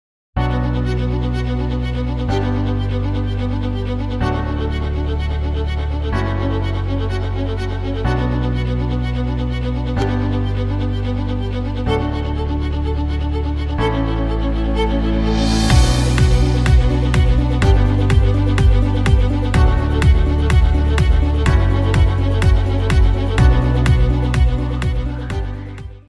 Nouvelle motorisation Skyactiv-X
Découvrez en avant première notre nouvelle motorisation Skyactiv-X qui combine les avantages du diesel et de l'essence